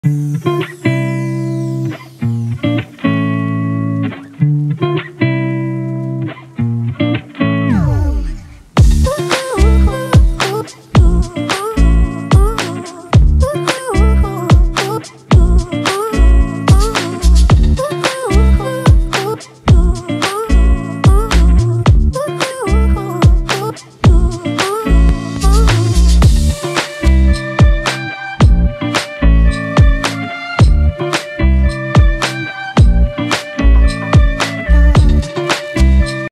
Phone screen magnifier
4 times screen HD magnification, comfortable without eye strain.
Prevent eye fatigue
Anti-radiation
Anti-blue light
Highly recommended by lots of people
The picture is super clear
Foldable storage